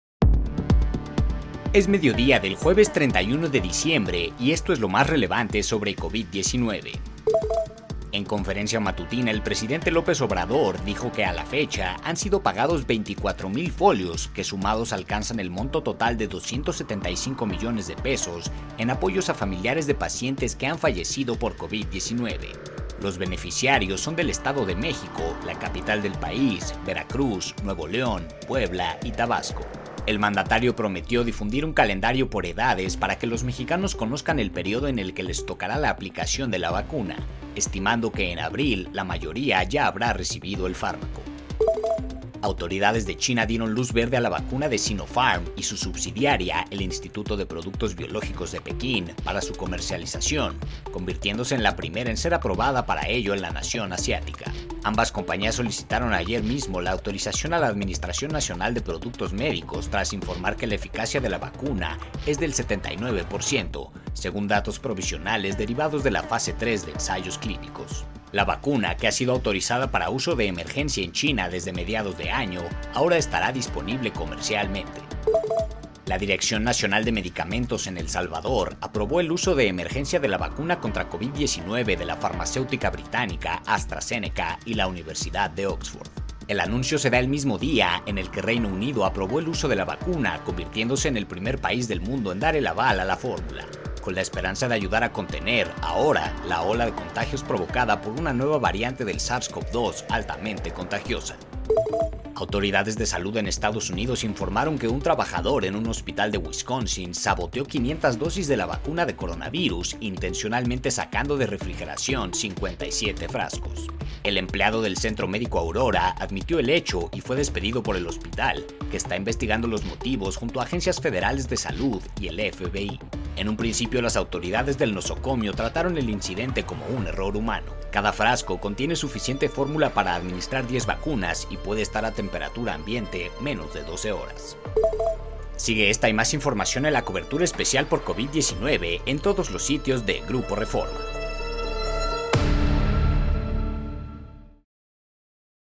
Reporte Covid-19 | 31 de diciembre
La suma de apoyos a familias en la pandemia; una nueva aprobación de vacuna en China y el sabotaje de 500 dosis de vacuna en EU.